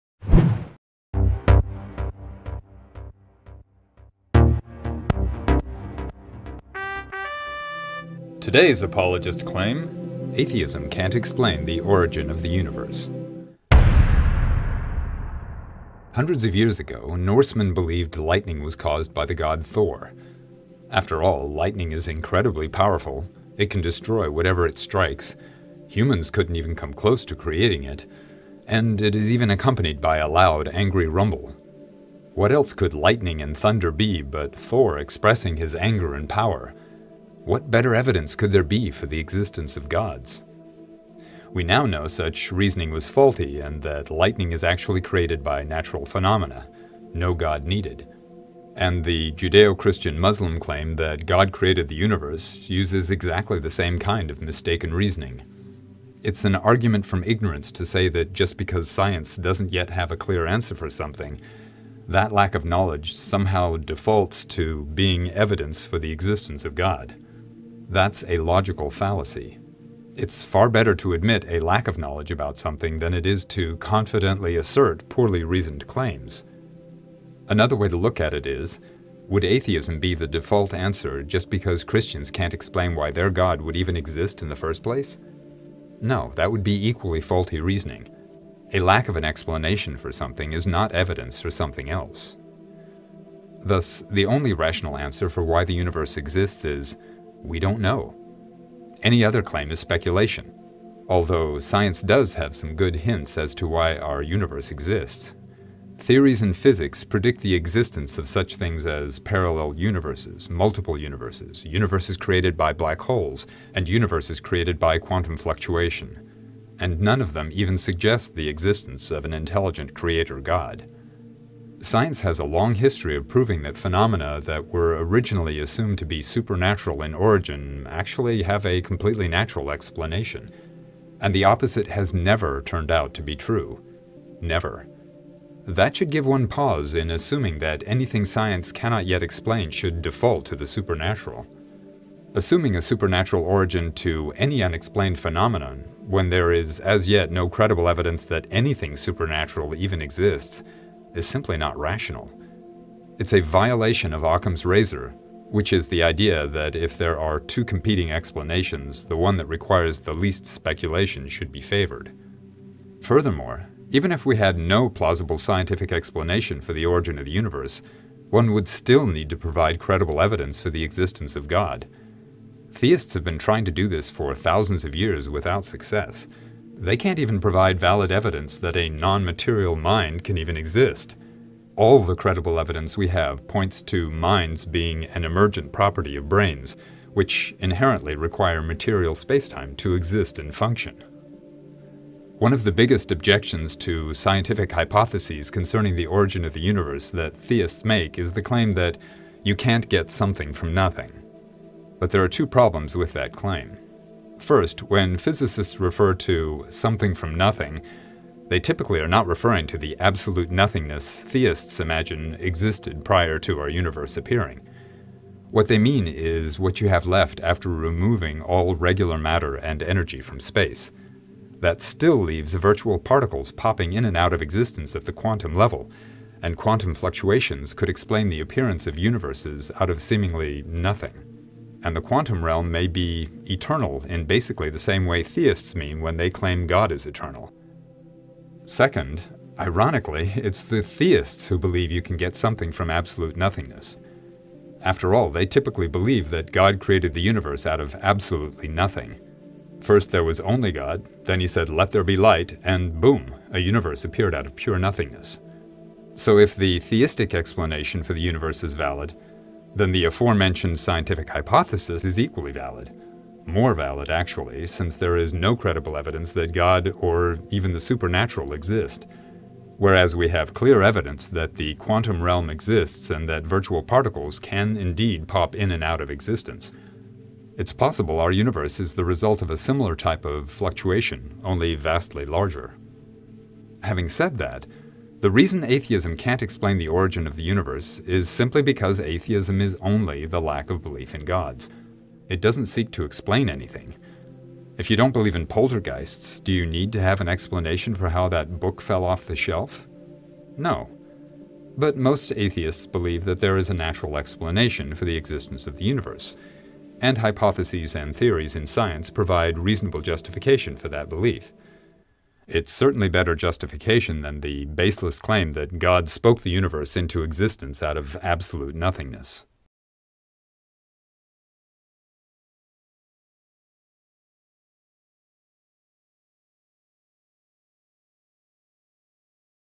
Atheist Answers #18: Atheism Can't Explain the Origin of the Universe
"Atheist Answers" responds to common claims made by Christian (and other religion) apologists. Today's video responds to the claim that atheism can't explain the origin of the universe.